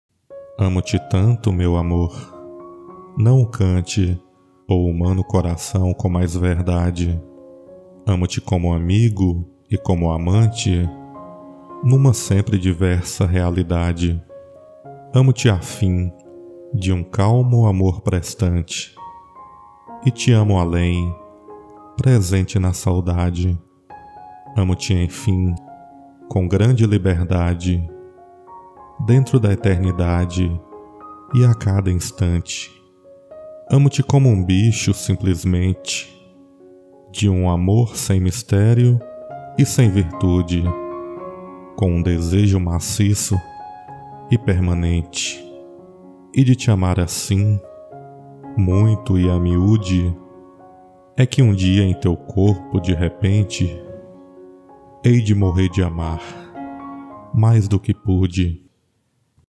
Soneto do amor total | Vinicius de Moraes
SONETO DO AMOR TOTAL

Amo-te tanto, meu amor… não cante
O humano coração com mais verdade…
Amo-te como amigo e como amante
Numa sempre diversa realidade

Amo-te afim, de um calmo amor prestante,
E te amo além, presente na saudade.
Amo-te, enfim, com grande liberdade
Dentro da eternidade e a cada instante.

Amo-te como um bicho, simplesmente,
De um amor sem mistério e sem virtude
Com um desejo maciço e permanente.

E de te amar assim muito e amiúde,
É que um dia em teu corpo de repente
Hei de morrer de amar mais do que pude.